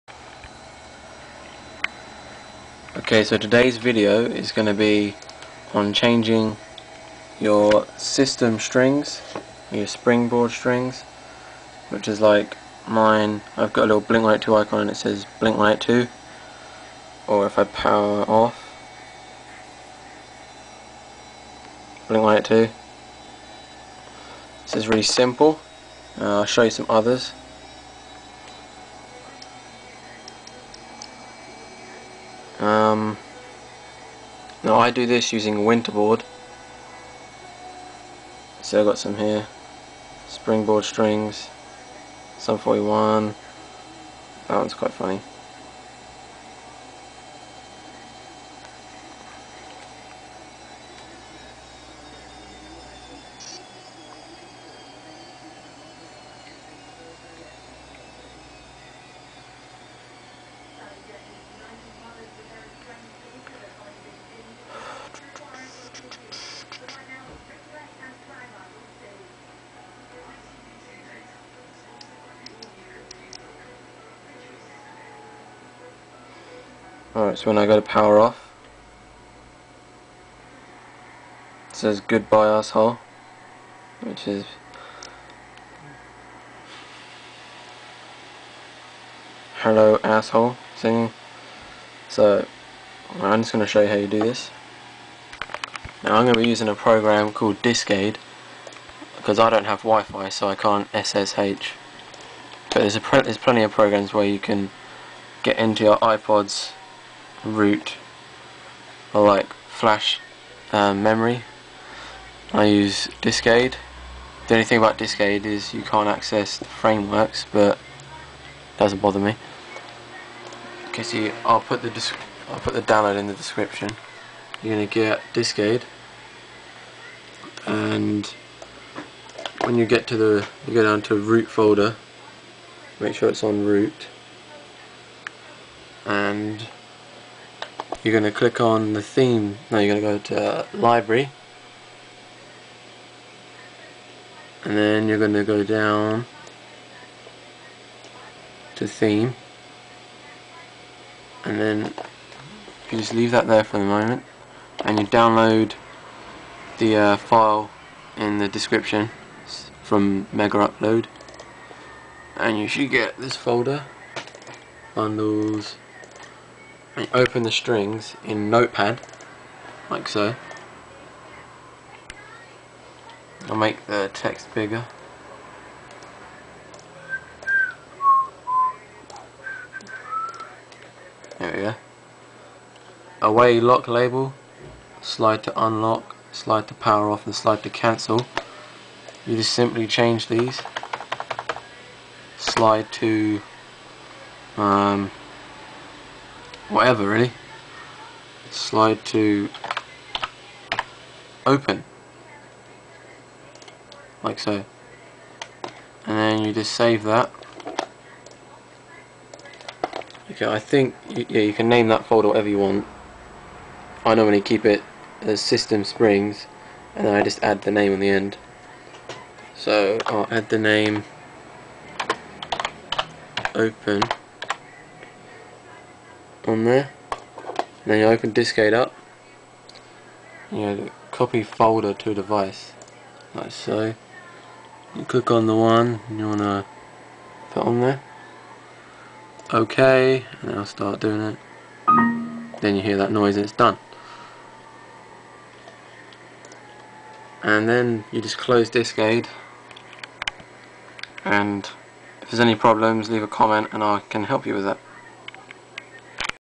CUSTOMISE SYSTEM STRINGS !!! (winterboard)
QUICK VIDEO ON HOW TO CHANGE SYSTEM STRINGS E.G, SLIDE TO UNLOCK OR SLIDE TO POWER OFF HERE ARE THE LINKS

2:disc aid or you can ssh!!

NEXT VIDEO TO COME WILL BE ON HOW TO MAKE CUSTOM SLIDE ICONS!! LIKE MY BLINK 182 ONE IN THE VIDEO!